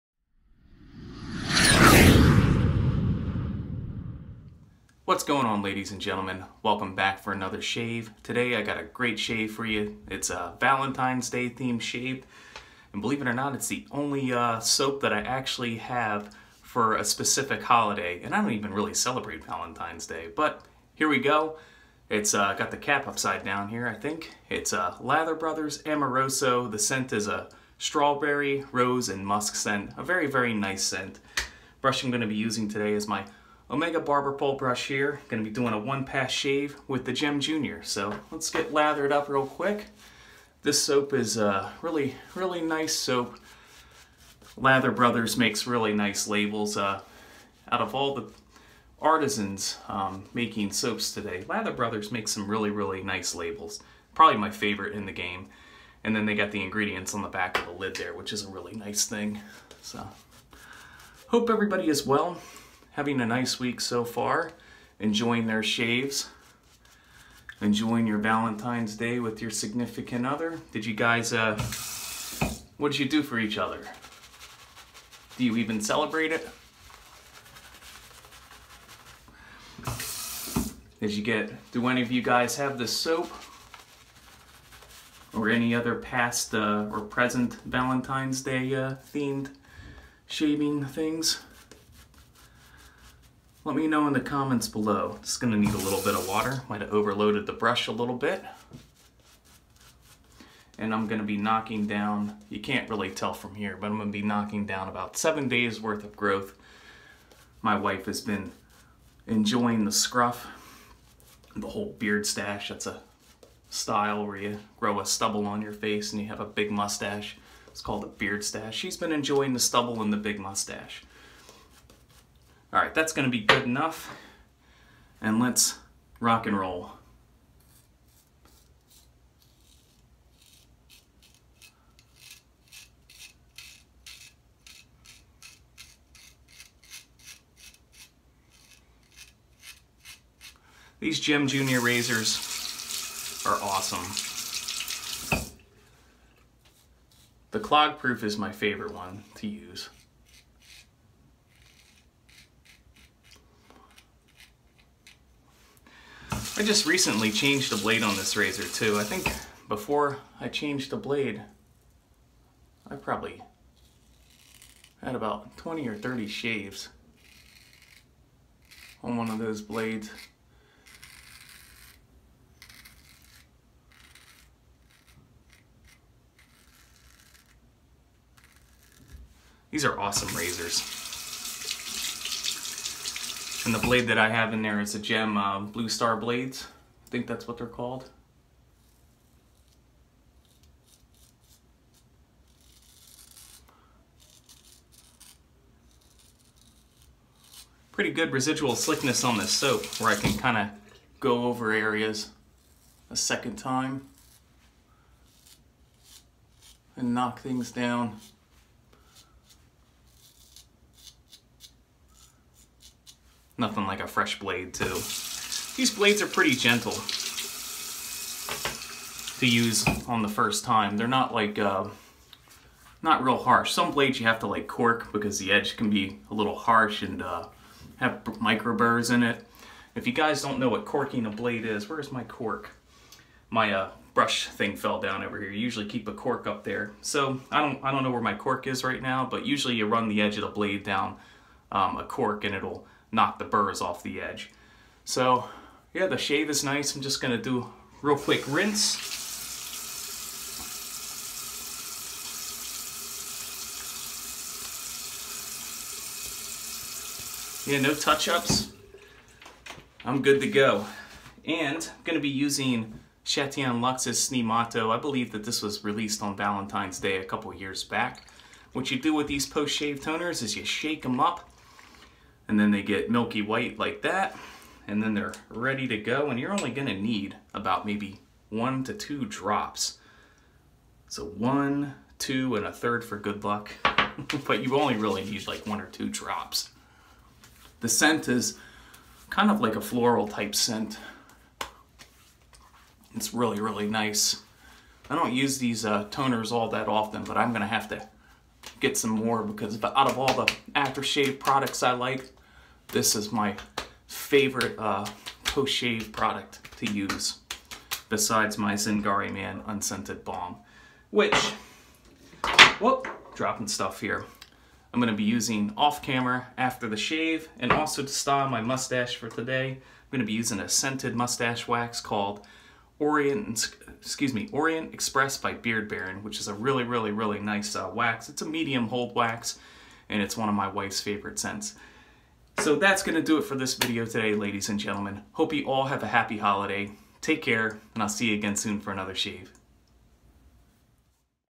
Valentines Day Shave
A Valentine’s Day Shave.

Lather Bros Amoroso Shave Soap
Gem JR Razor
Omega Hi Brush
Chatillon Lux Sni Moto Post Shave Toner
Beard Baron Mustache Wax